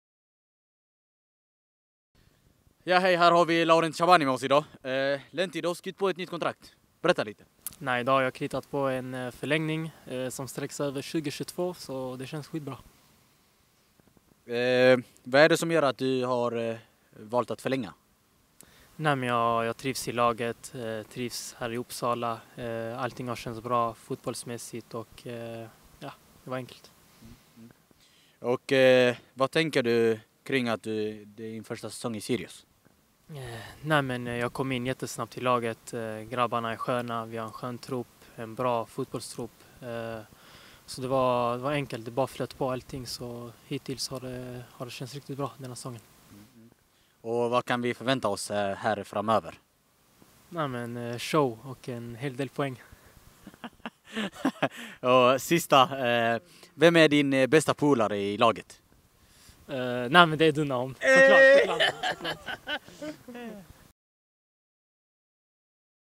Laorent Shabani förlänger sitt kontrakt till 2022
Idag har Laorent Shabani signerat ett nytt avtal med Sirius Fotboll. Det nya kontraktet sträcker sig över säsongen 2022.